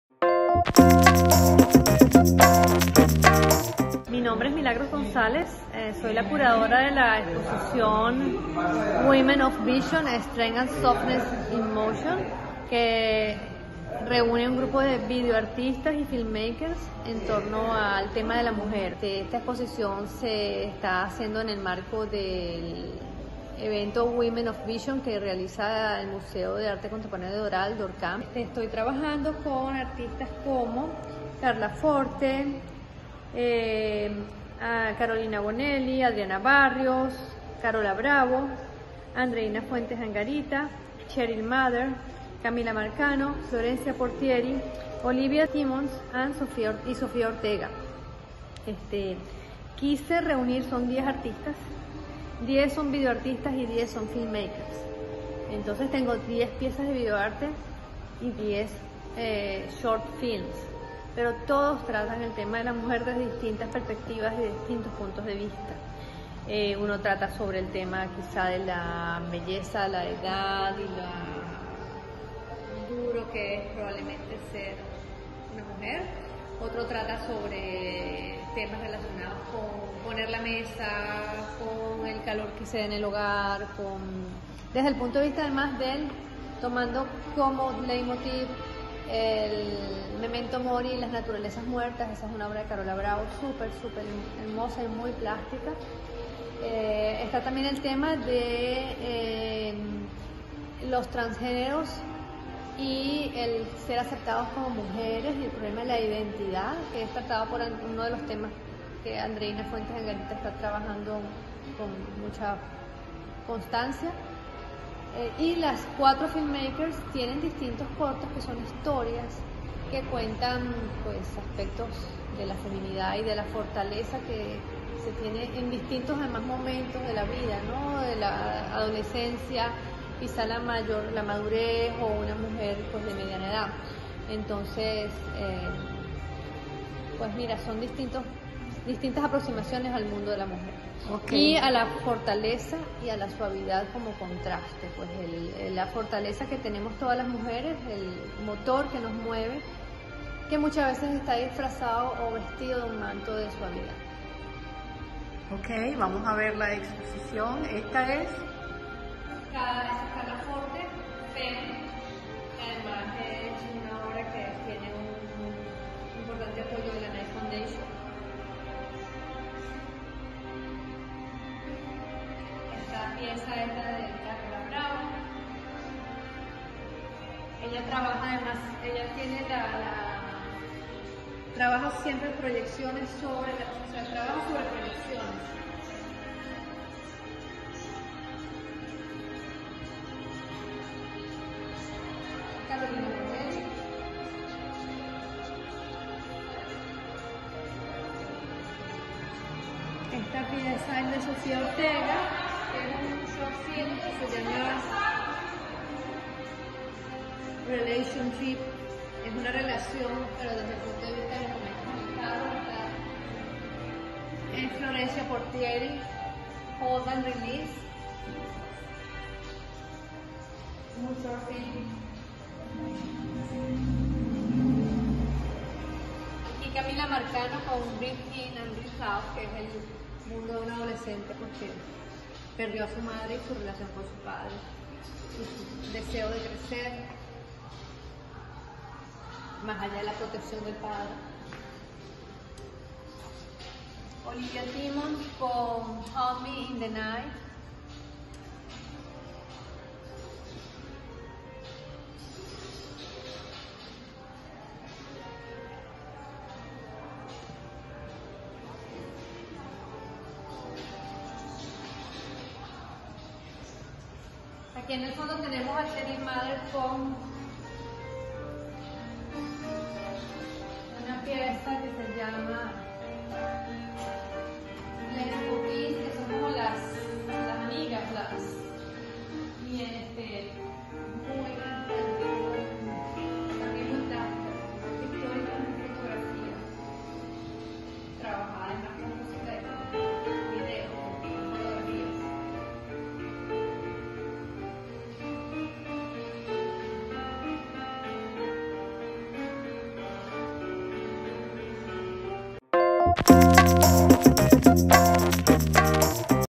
Milagros González_Entrevista_“Women of Vision. Strength and Softness in Motion”_DORCAM
Entrevista a Milagros González, curadora de la exposición “Women of Vision. Strength and Softness in Motion”, Short Films, and Video Art Exhibition. Doral Contemporary Art Museum - DORCAM
CityPlace Doral, Miami

Artistas participantes: Adriana Barrios, Carolina Bonnelly, Carola Bravo, Carla Forte, Andreína Fuentes Angarita, Cheryl Maeder, Camila Marcano, Florencia Porteri, Olivia Timmons, Sofía Ortega.

Nanchi Farfant Art Report
Marzo, 2022